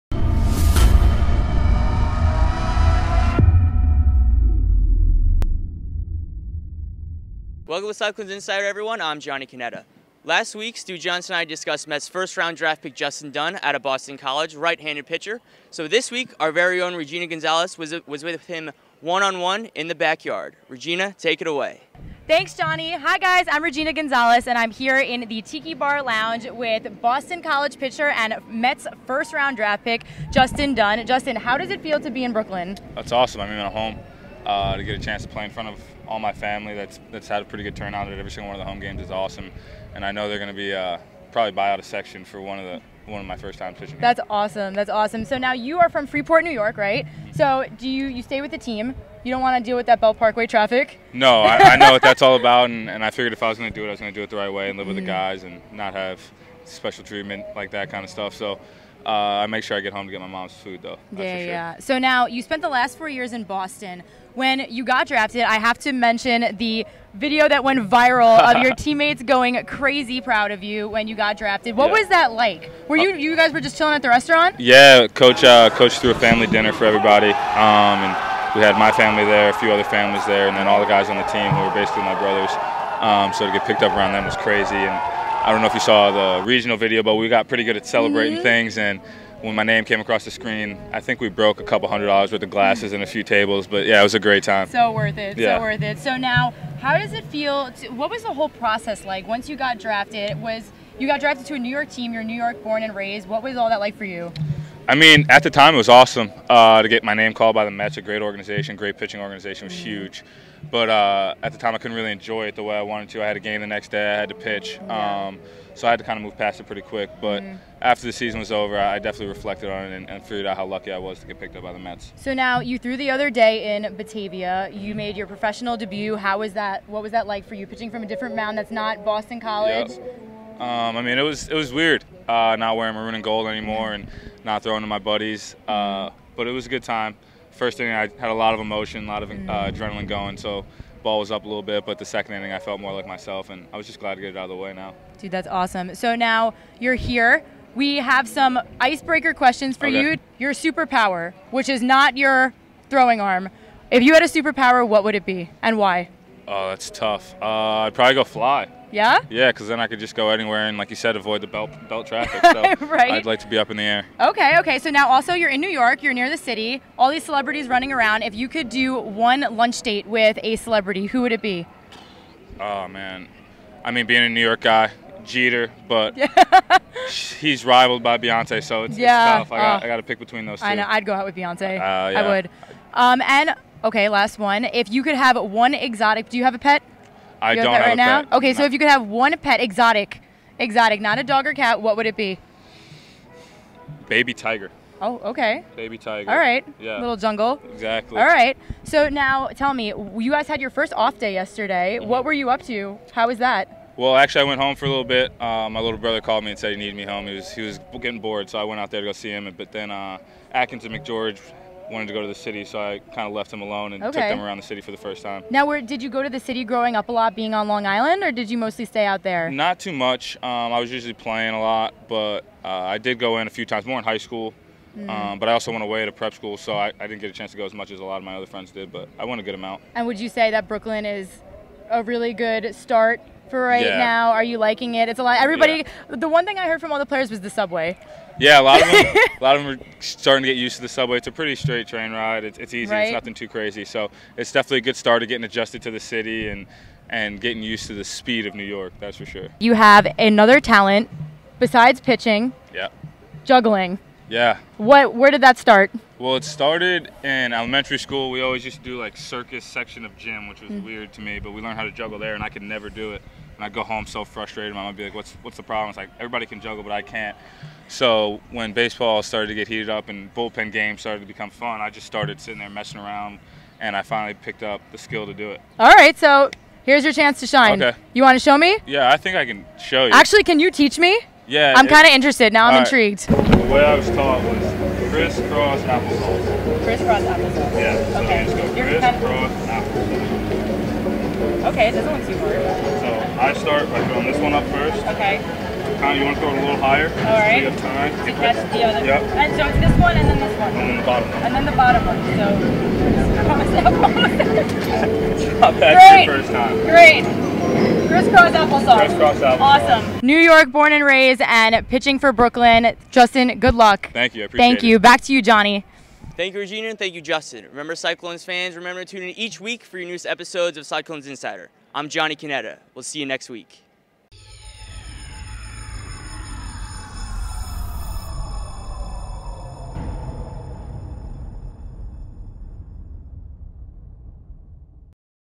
Cyclones Insider - Week 4 - July 13th
This week, we sit down with Mets 1st Round Draft Pick Justin Dunn.